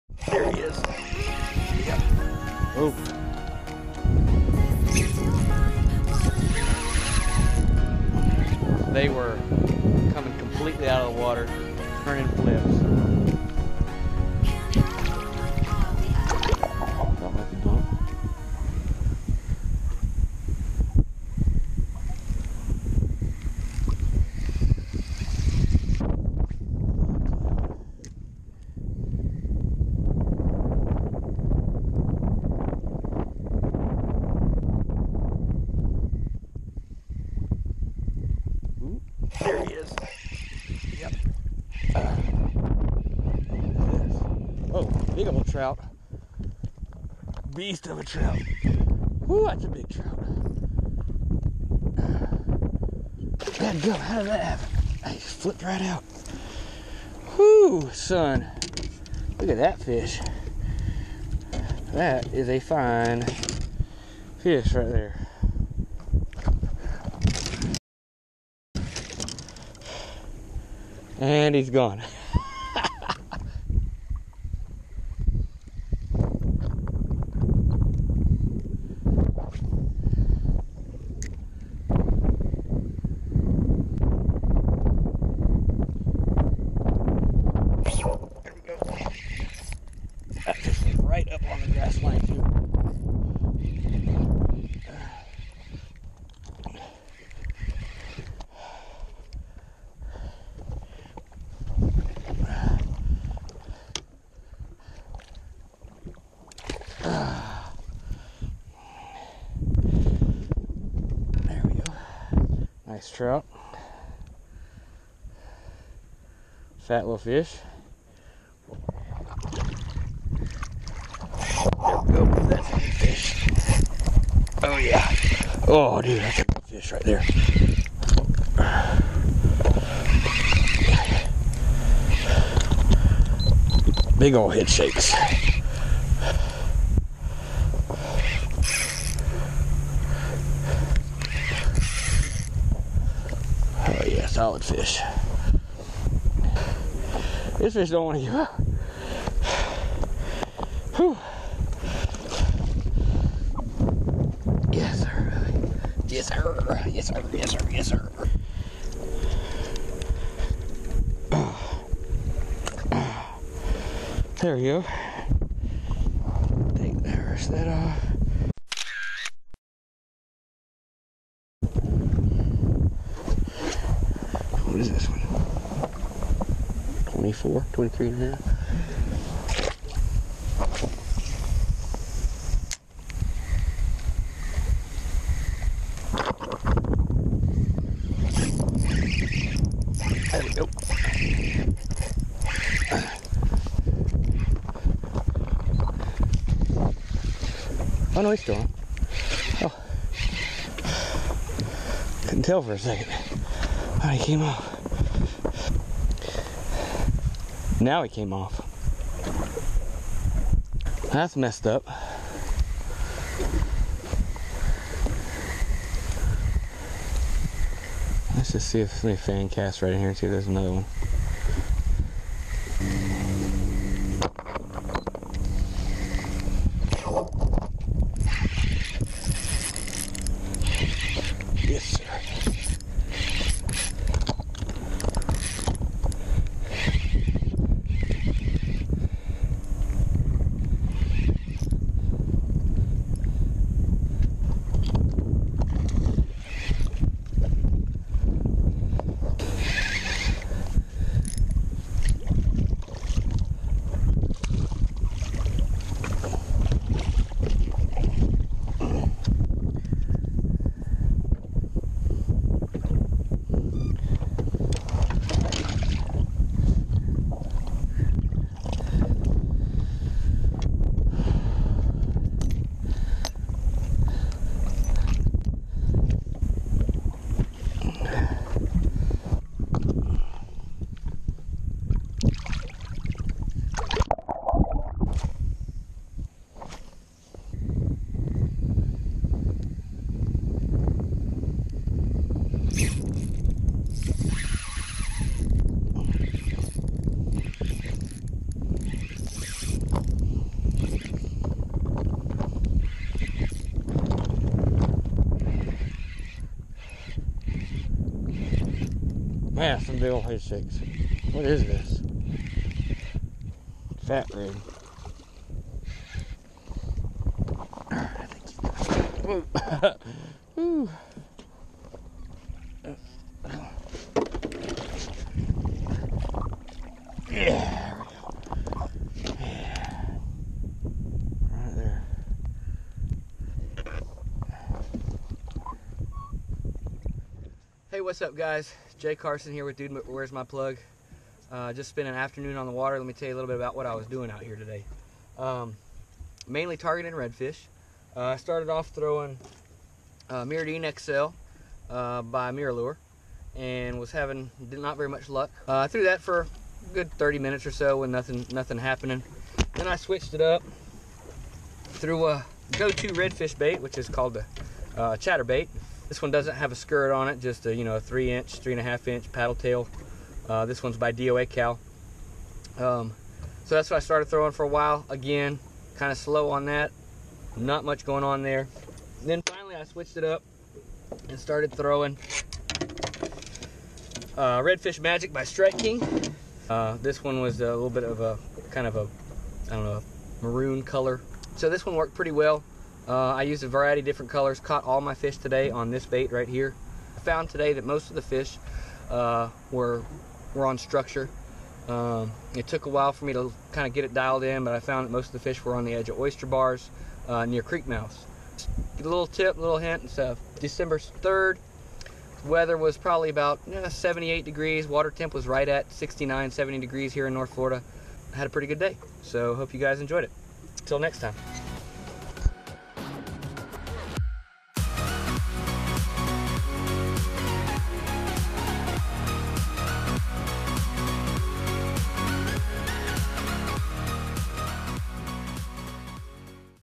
Jay's Search for Redfish/Trout around the Econfina River!
Jay Carson on a solo mission out of the Econfina River here in North Florida.